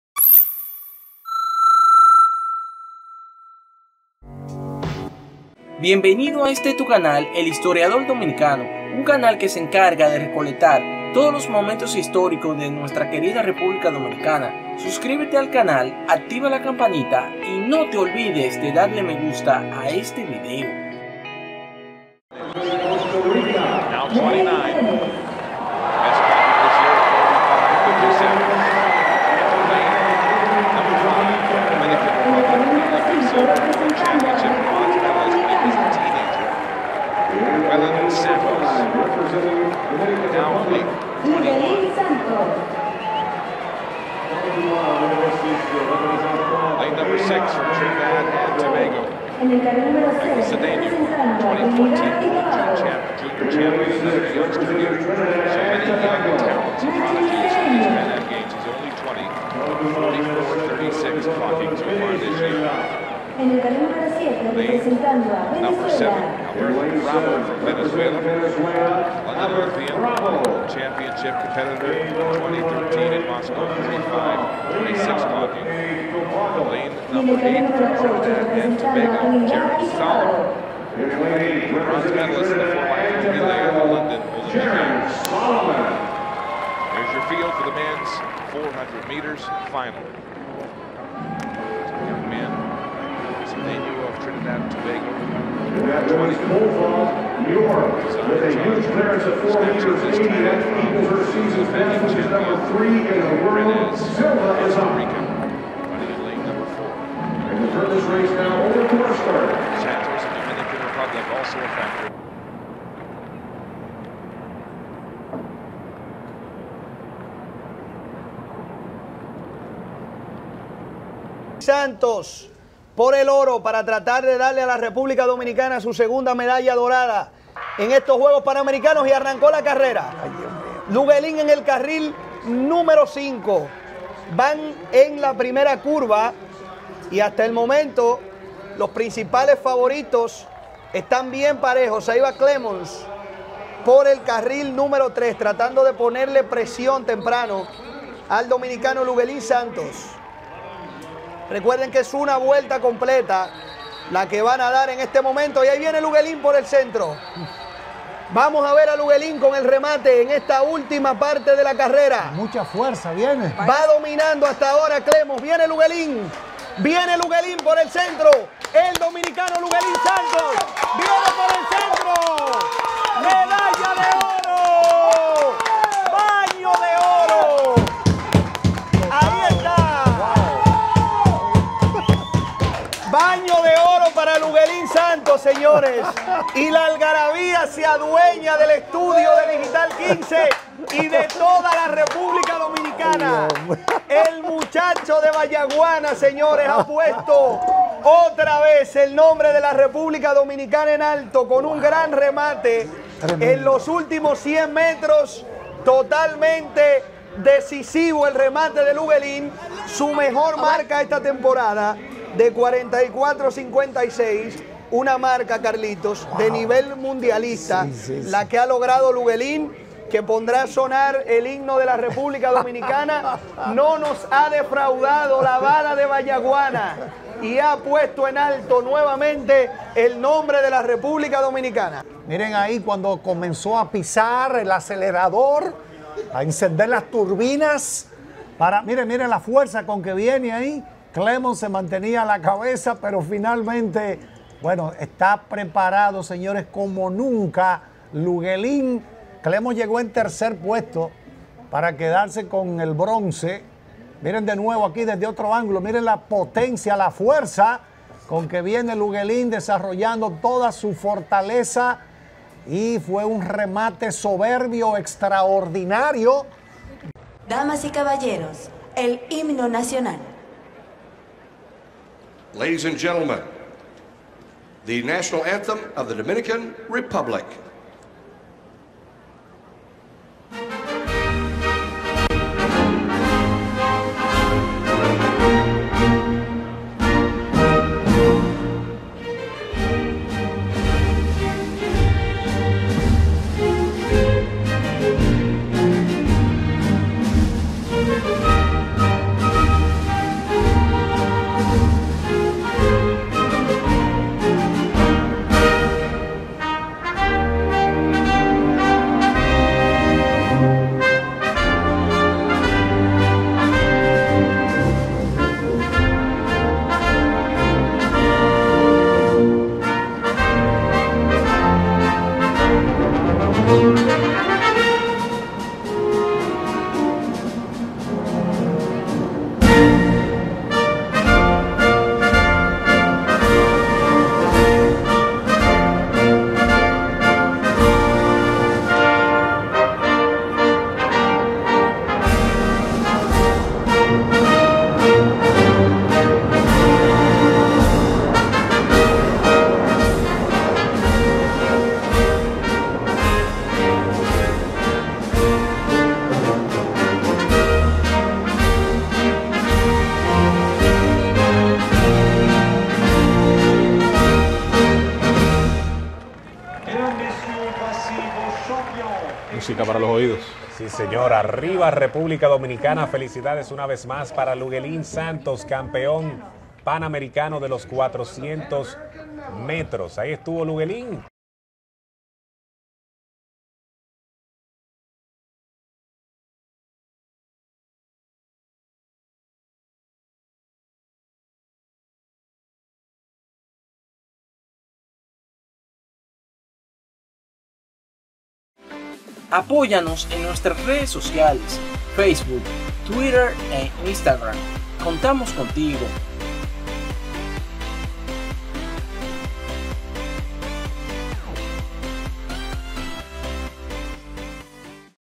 Luguelín Santos 🇩🇴 gana la medalla de oro 🥇 en los Juegos Panamericanos │Toronto 2015│
El 23 de julio del año 2015, el velocista dominicano Luguelín Santos gano la medalla de oro en los 400 metros planos en los Juegos Panamericanos de Toronto,  Santos hizo un tiempo de 44.56 segundos para ganar la presea dorada.